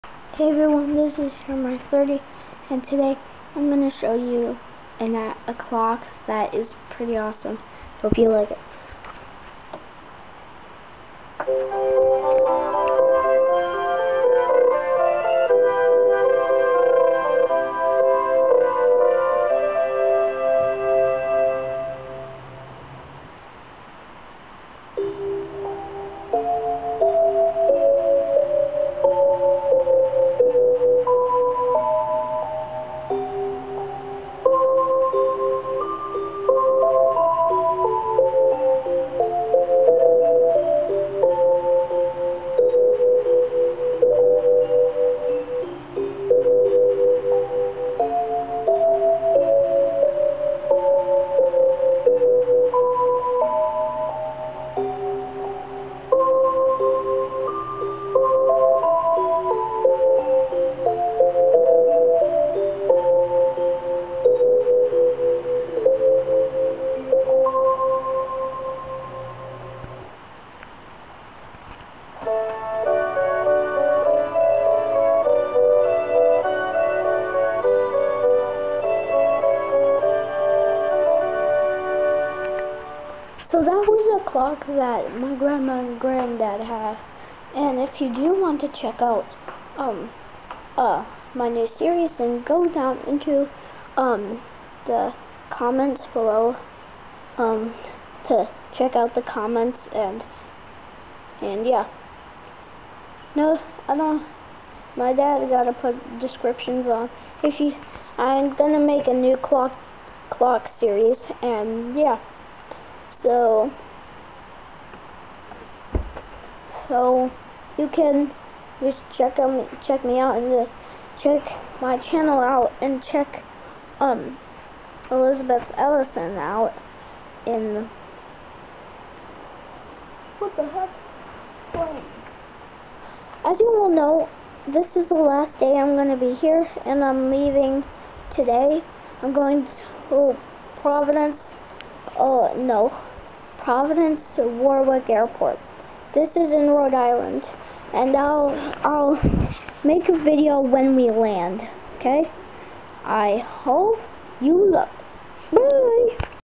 Clock Showcase
Im Testing a musical clock its so awesome